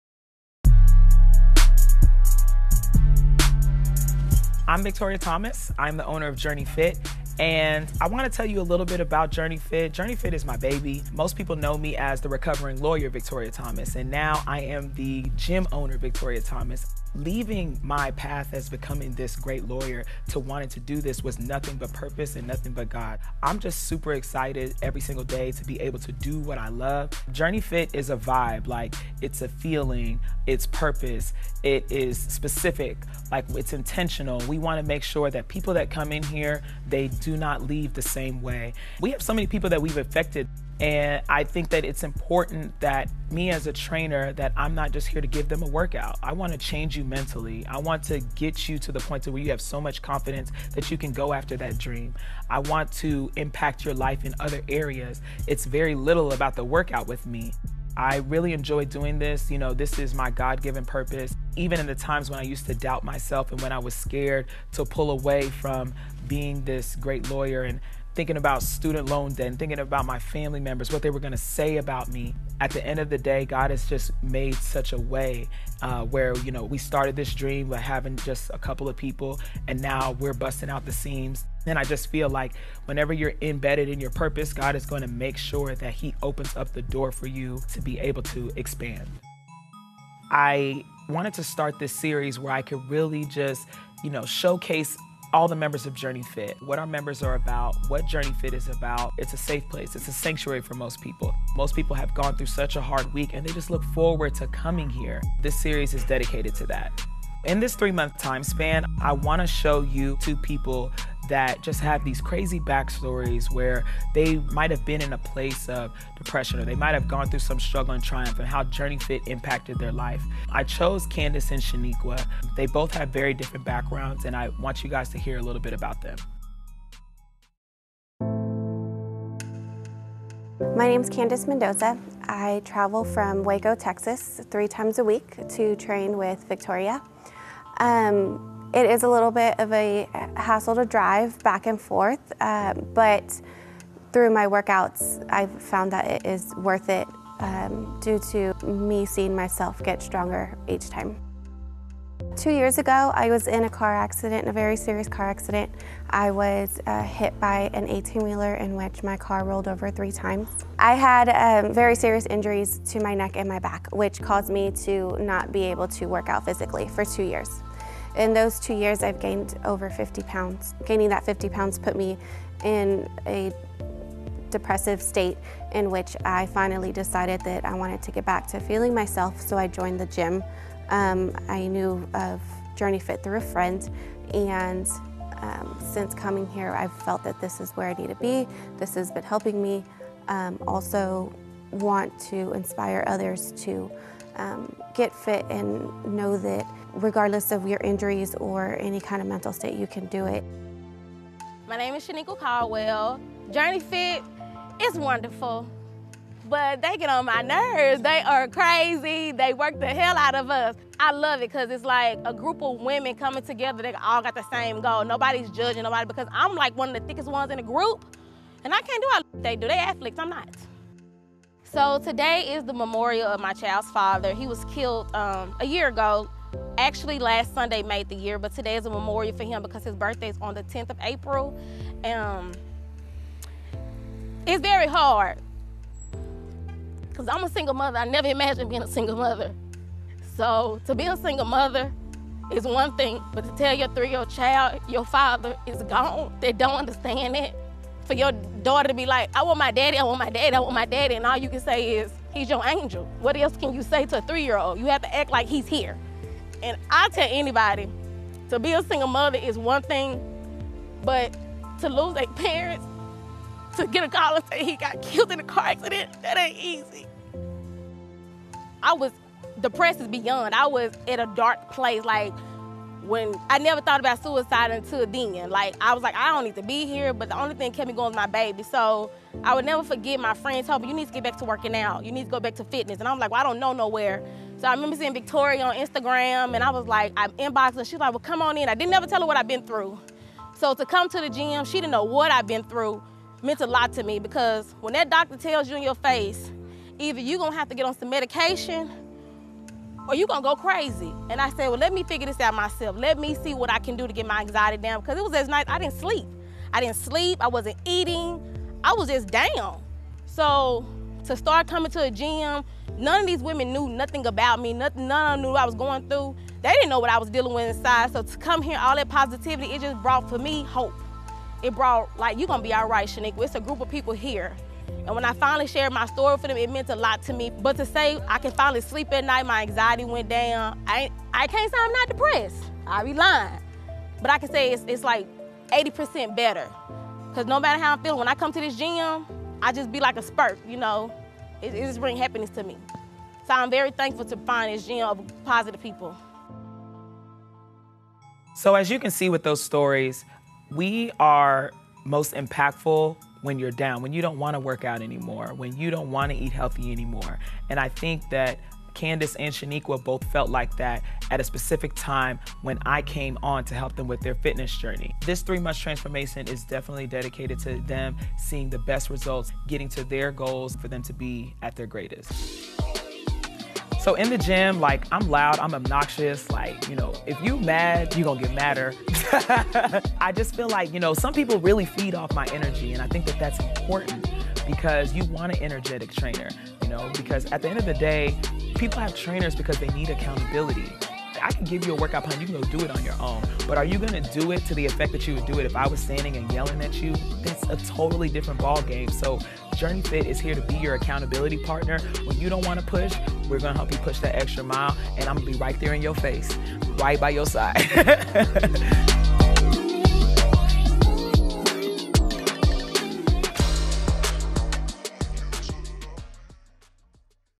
Season 1 - Episode 1 - Strength of a woman
Introduction to JOURNEYFIT, the members, and owner Victoria Thomas.